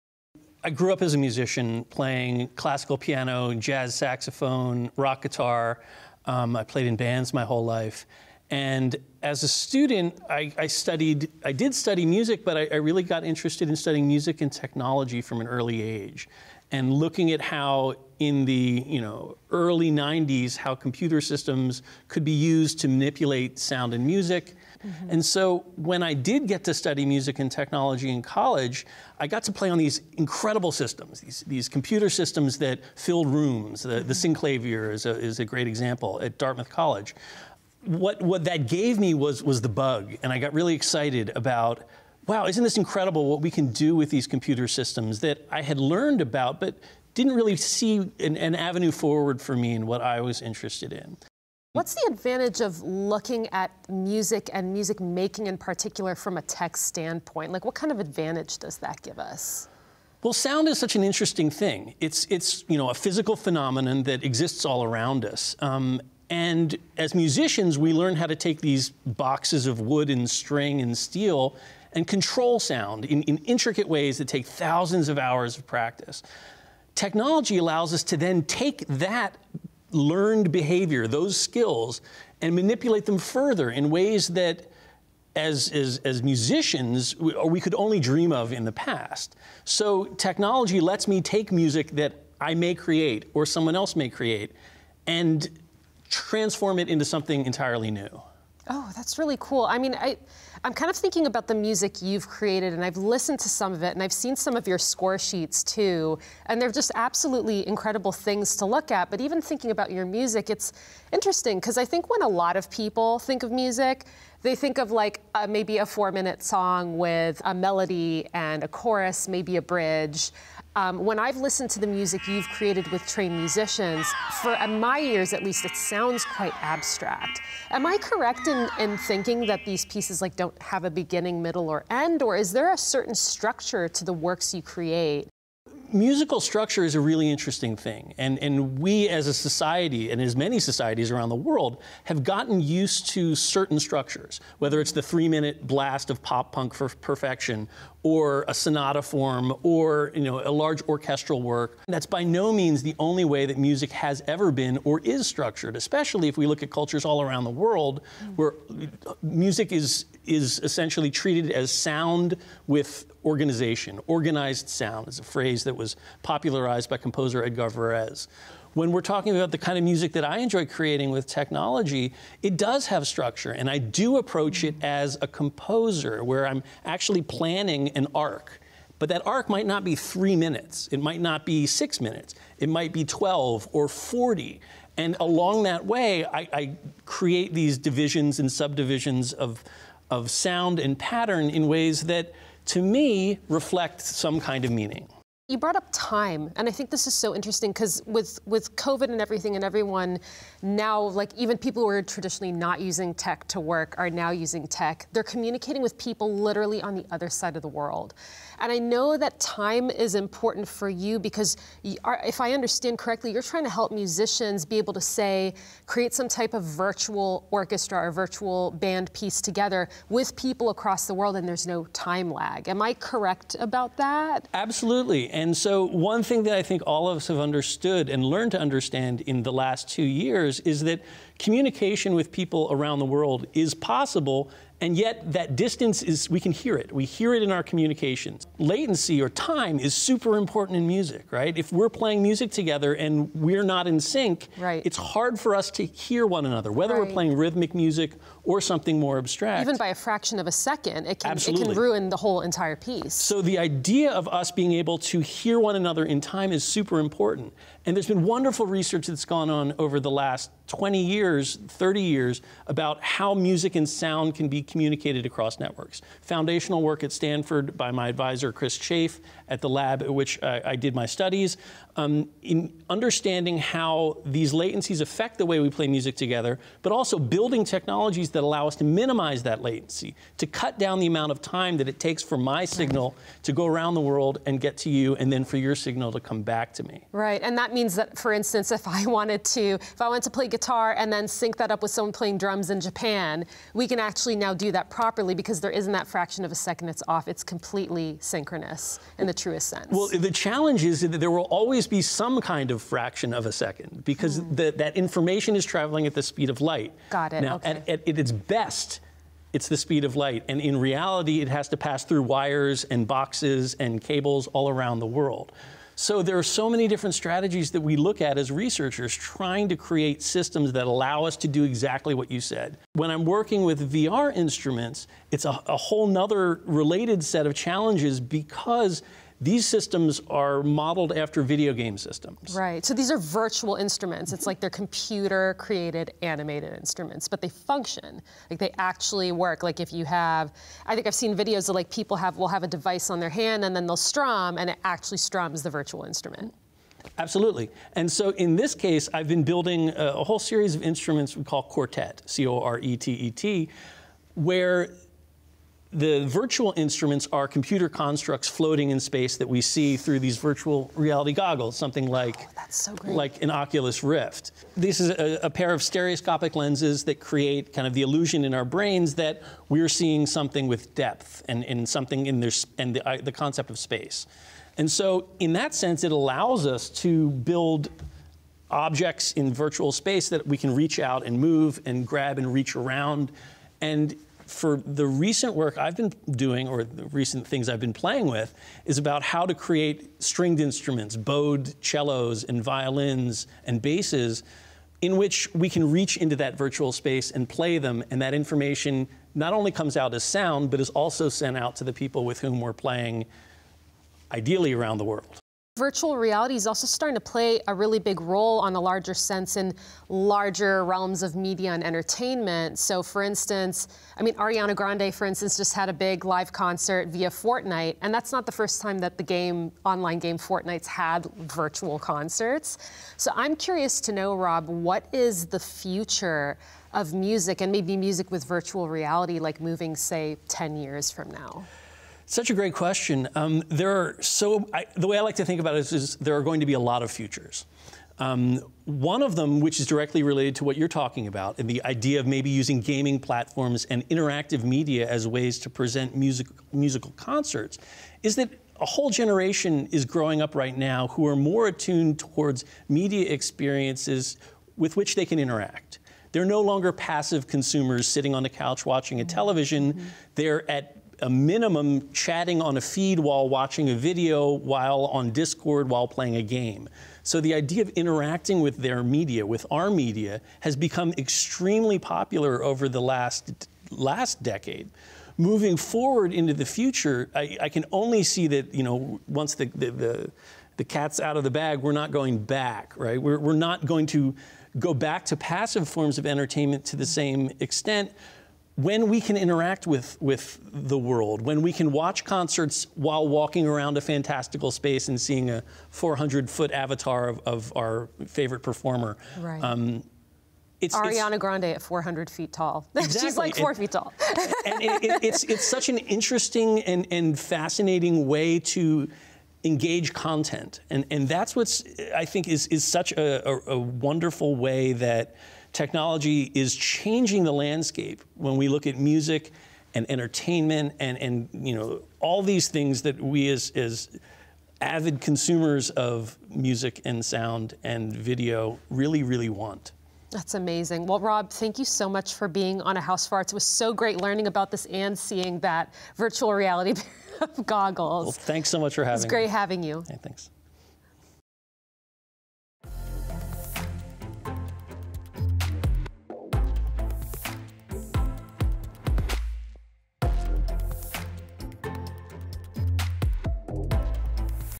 The Future of Music with Rob Hamilton | AHA! A House for Arts
Rob Hamilton is an Associate Professor of Music and Media at Rensselaer Polytechnic Institute. His expertise lies in the intersection of technology, sound, and music. What does the future of music look like? Lara Ayad finds out.